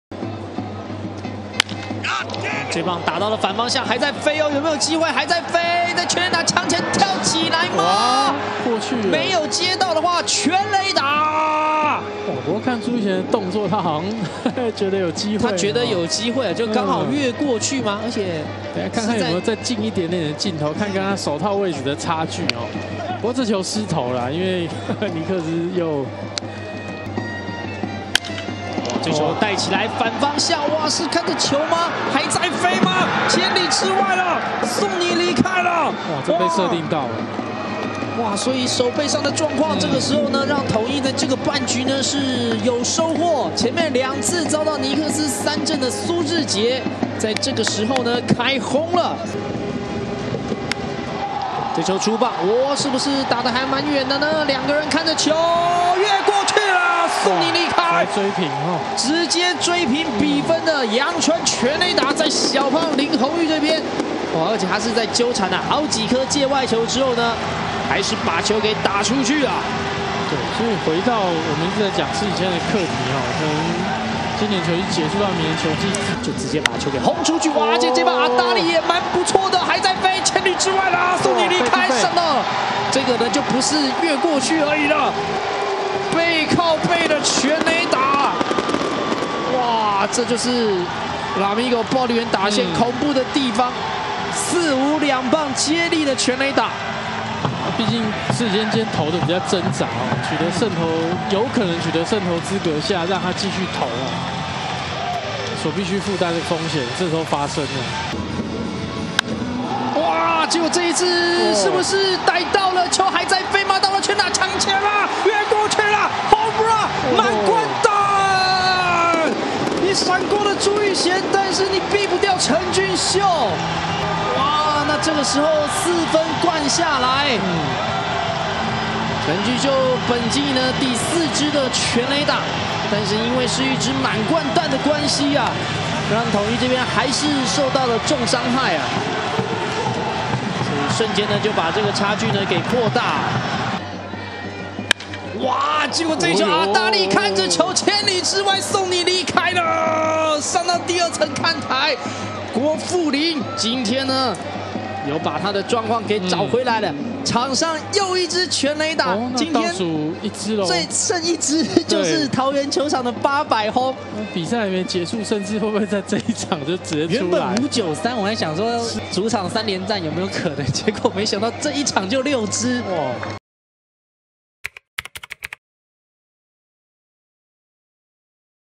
【職棒30精彩好球】06/14 Lamigo桃猿、統一獅煙火秀！單場6發全壘打錦集
CPBL中華職棒：Lamigo桃猿、富邦悍將、統一獅、中信兄弟主場賽事